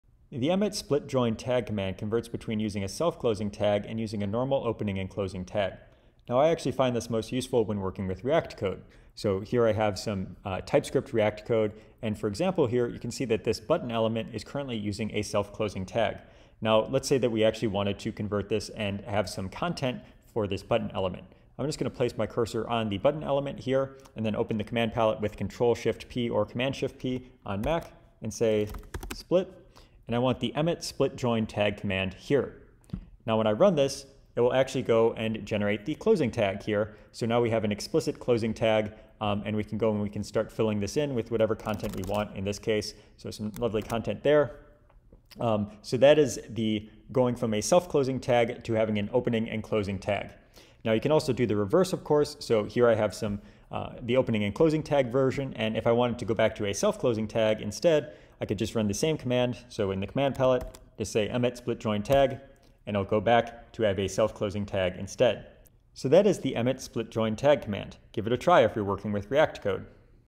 VS Code tips — Splitting and joining html tags using Emmet
Today's VS Code command: Emmet split/join tag

Convert between using an opening+closing tag in html and a self-closing tag.

I personally find this most useful when working with ReactJS